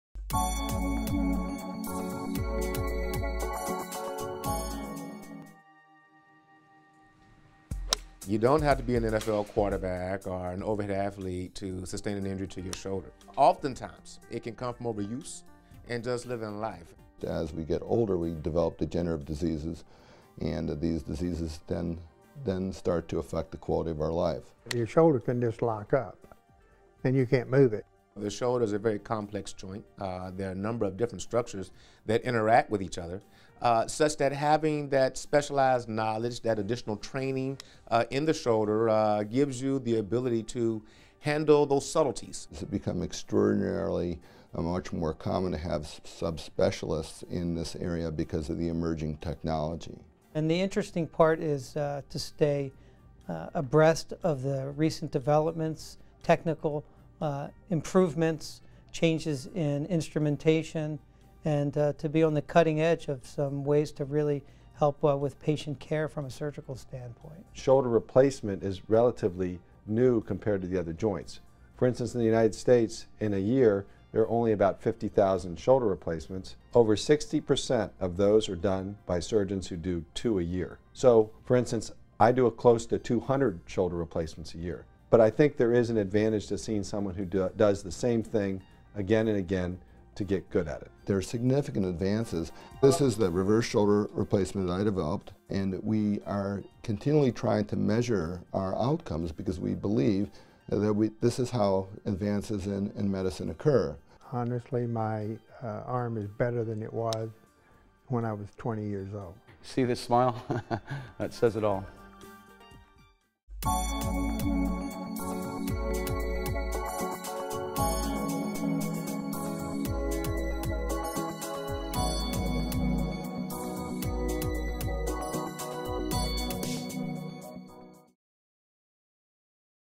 Shoulder and Elbow Specialty - Florida Orthopaedic Institute
You don't have to be an NFL quarterback or an overhead athlete to sustain an injury to your shoulder. Oftentimes it can come from over use and does live in life. As we get older, we develop degenerative diseases and these diseases then start to affect the quality of our life. Your shoulder condition lock up and you can't move it.

Well, the shoulder is a very complex joint. There are a number of different structures that interact with each other such that having that specialized knowledge, that additional training in the shoulder gives you the ability to handle those subtleties.

It's become extraordinarily much more common to have subspecialists in this area because of the emerging technology. And the interesting part is to stay abreast of the recent developments, technical improvements, changes in instrumentation, and to be on the cutting edge of some ways to really help with patient care. From a surgical standpoint, shoulder replacement is relatively new compared to the other joints. For instance, in the United States, in a year, there are only about 50,000 shoulder replacements. Over 60% of those are done by surgeons who do two a year. So for instance, I do a close to 200 shoulder replacements a year, but I think there is an advantage to seeing someone who does the same thing again and again to get good at it. There are significant advances. This is the reverse shoulder replacement I've developed, and we are continually trying to measure our outcomes because we believe that we, this is how advances in medicine occur. Honestly, my arm is better than it was when I was 20 years old. See this smile that says it all.